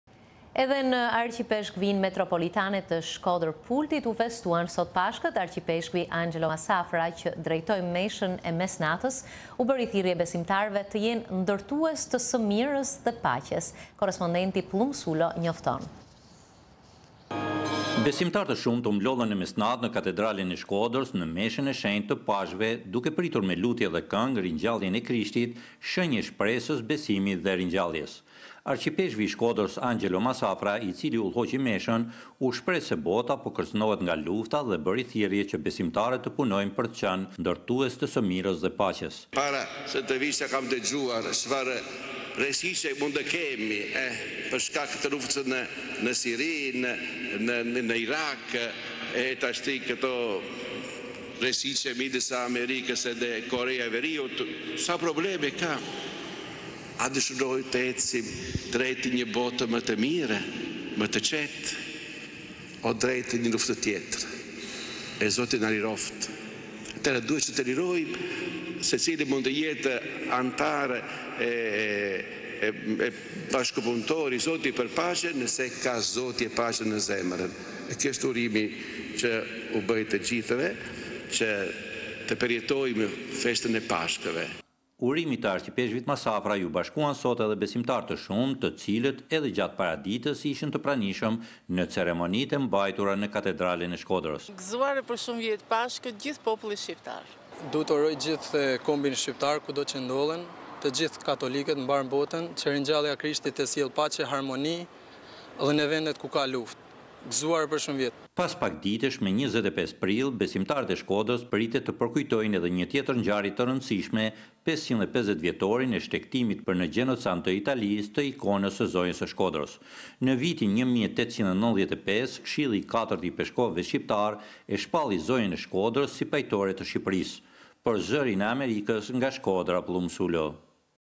Besimtarët katolikë kremtojnë Pashkën
Edhe në Arqipeshkvinë Metropolitane të Shkodrës-Pultit u festuan sot Pashkët. Arqipeshkvi Angelo Massafra, që drejtoi meshën e mesnatës, u bëri thirrje besimtarëve që të jenë ndërtues të së mirës dhe paqes.